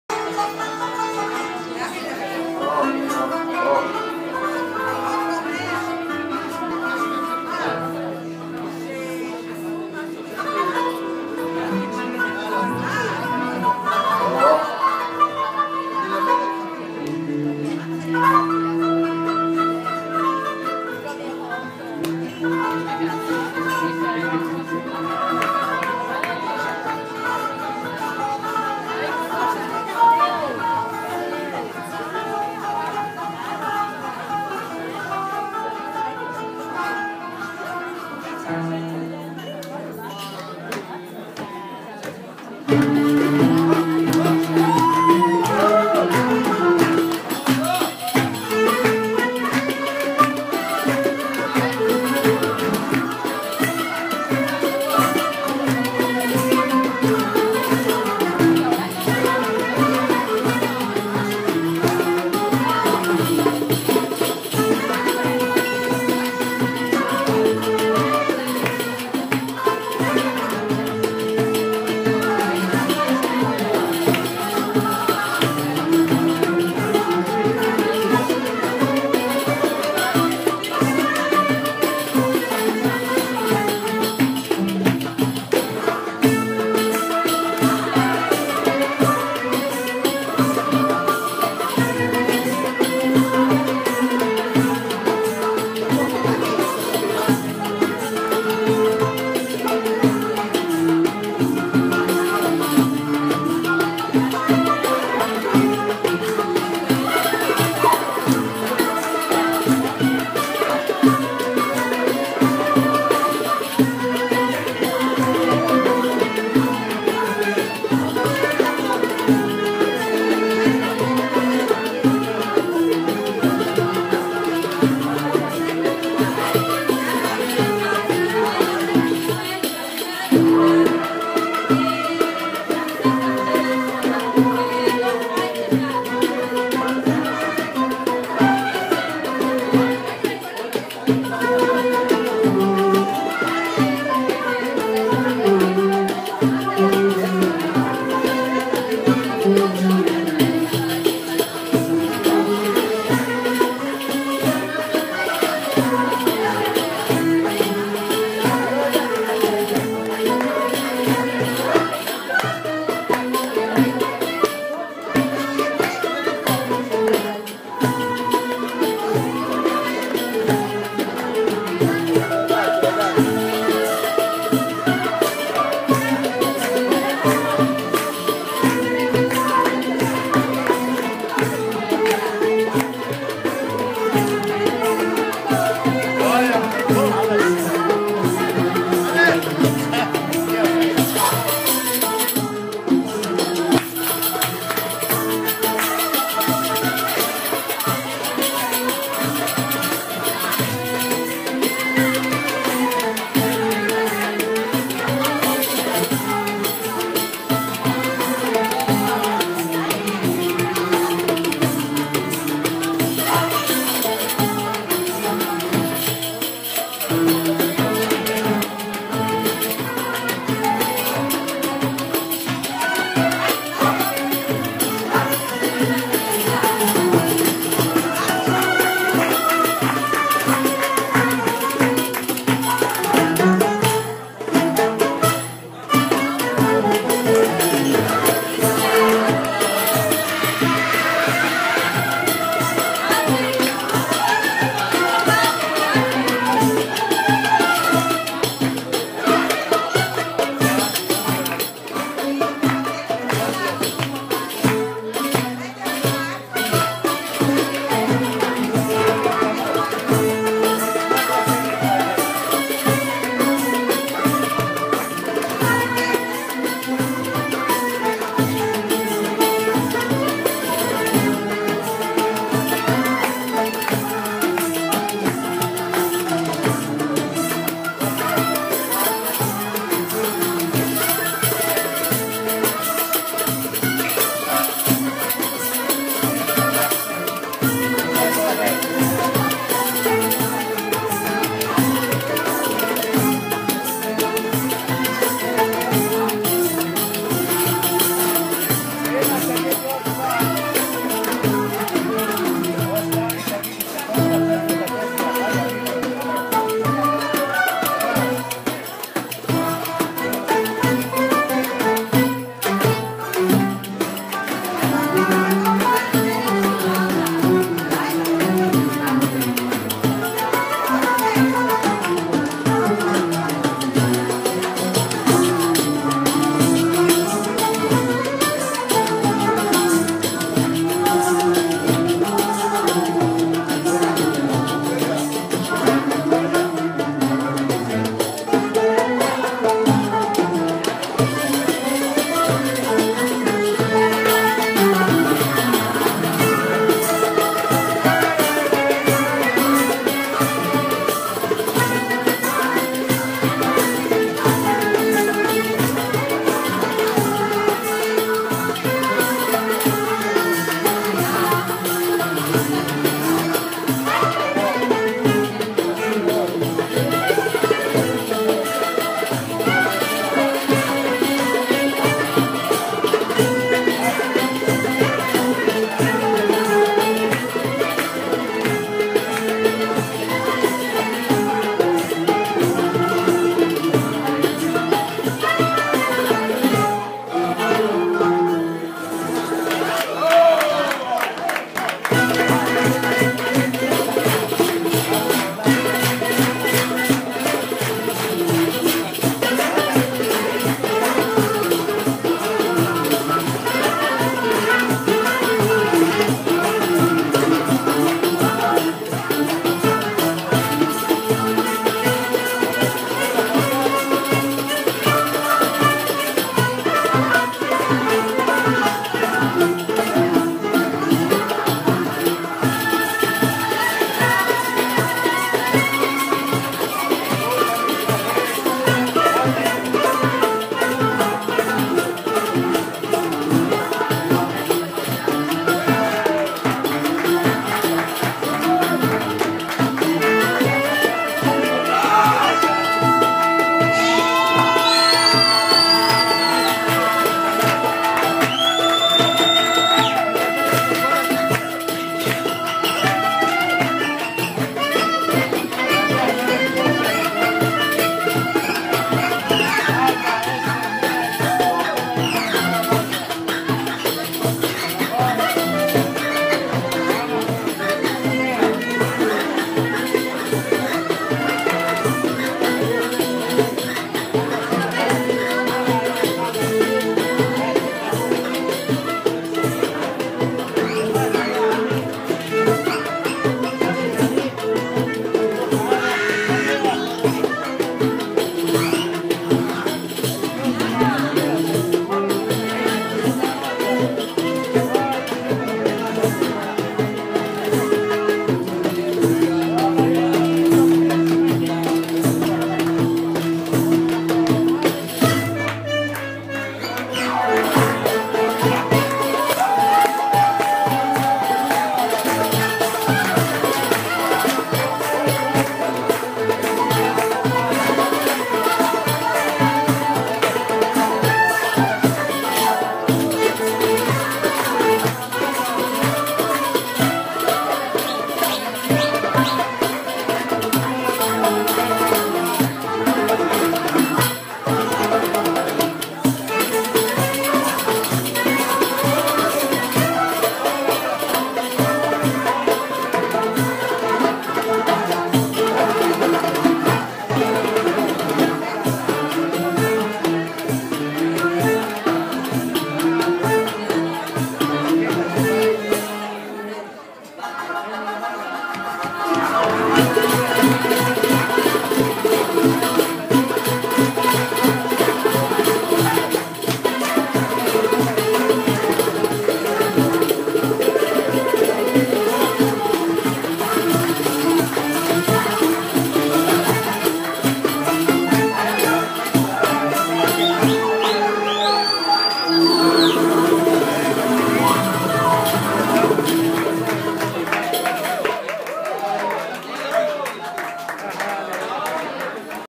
Harel Shachal at Cafe Shefa, Yafo, Israel, Apr 7, 2014, part 3
3rd segment by Harel Shachal and the Ottomans from two sets at Cafe Shefa, Apr 7, 2014. There is some choppiness between segments as I sometimes lost the image and had to start recording again in the middle of a piece. The band was hot, the audience was extremely lively, and recording conditions were not good. I hope this conveys some of the excitement of the performance. You can find out more about the weekly session on Facebook